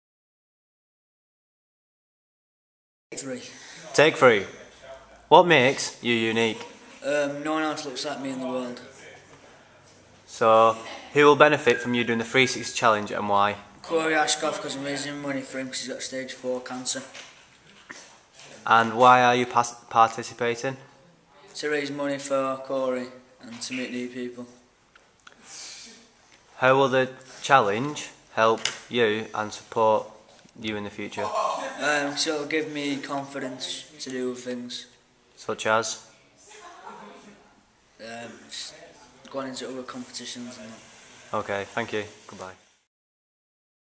Mojo's Video blog
Mojo (Jack Harvey) is one of the 16 cyclists taking up the Big 360° Challenge to cycle around the county of Lancashire this August 2011.  Be sure to catch up on how he gets on during the cycle:)